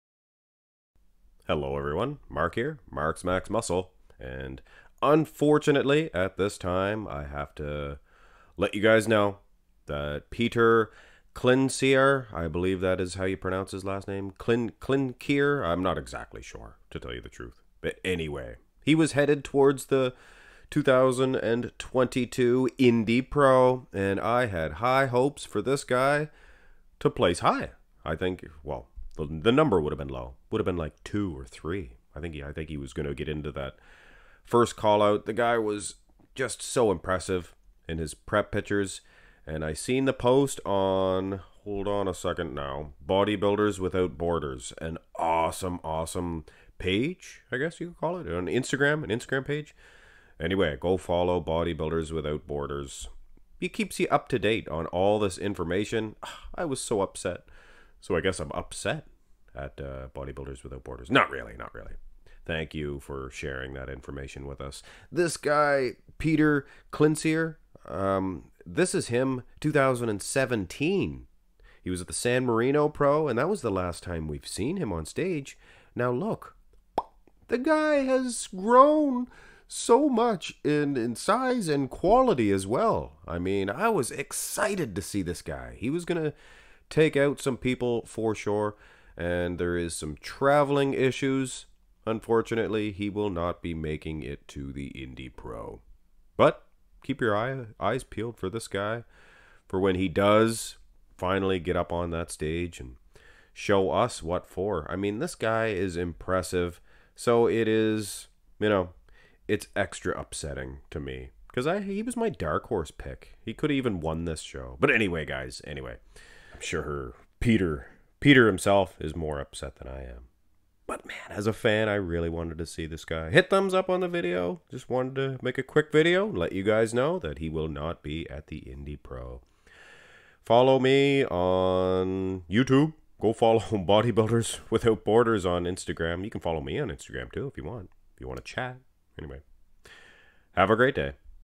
Petar Klancir Out from The Indy Pro
This is a quick update on the 2022 Indy Pro. At least one guy has dropped out from this even due to travel issues. Petar Klancir is out this time, but rest assure, he will be back sooner than later to make a name for himself.